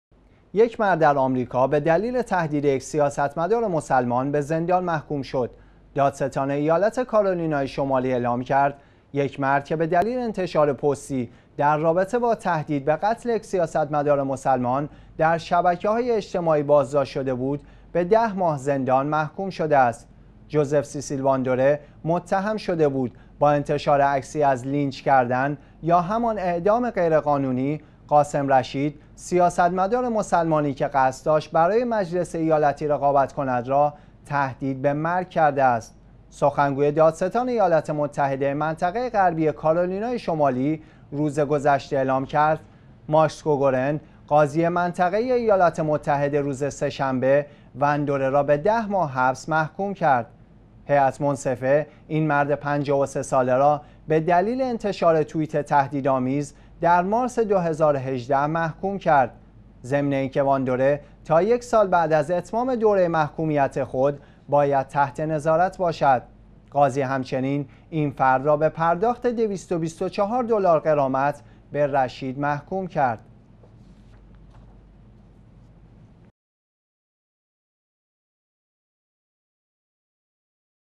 مجازات زندان برای تهدید سیاستمدار مسلمان به قتل
یک مرد در آمریکا به دلیل تهدید یک سیاستمدار مسلمان به زندان محکوم شد.
دادستان ایالت کارولینای شمالی اعلام کرد: یک مرد که به دلیل انتشار پستی در رابطه با تهدید به قتل یک سیاستمدار مسلمان در شبکه‌های اجتماعی بازداشت شده بود، به 10 ماه زندان محکوم شده است.
جوزف سِسیل واندوره (Joseph Cecil Vandevere)، متهم شده بود با انتشار عکسی از لینچ کردن - یا همان اعدام غیرقانونی- قاسم رشید سیاستمدار مسلمانی که قصد داشت برای مجلس ایالتی رقابت کند، را تهدید به مرگ کرده است.
سخنگوی دادستان ایالات متحده منطقه غربی کارولینای شمالی روز گذشته اعلام کرد: ماکس کوگورن جونیور قاضی منطقه‌ای ایالات متحده، روز سه‌شنبه وندوره را به ده ماه حبس محکوم کرد. هیئت منصفه این مرد ۵۳ ساله را به دلیل انتشار توییت تهدیدآمیز در مارس ۲۰۱۸، محکوم کرد.
ضمن اینکه واندوره تا یکسال بعد از اتمام دوره محکومیت خود باید تحت نظارت باشد.
قاضی همچنین این فرد را به پرداخت ۲۲۴ دلار غرامت به رشید محکوم کرد.